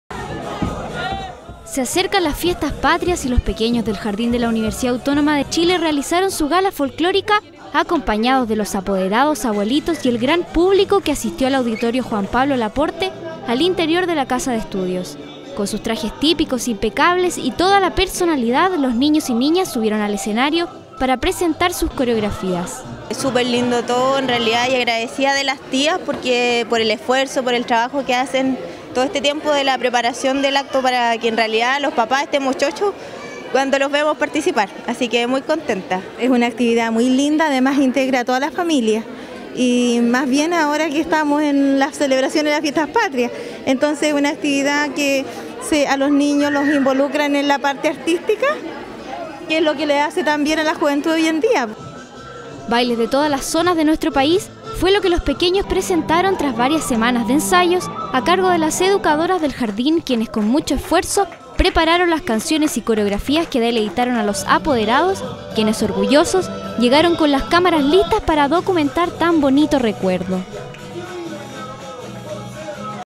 Jardín de la Universidad Autónoma realizó gala folclórica
Esta tarde los niños y niñas del jardín de la Universidad Autónoma de Chile, presentaron su gala folclórica en el auditorio Juan Pablo Laporte, el cual abrió sus puertas para recibir al amplio público que llegó para deleitarse de la presentación que los pequeños junto a sus educadoras prepararon.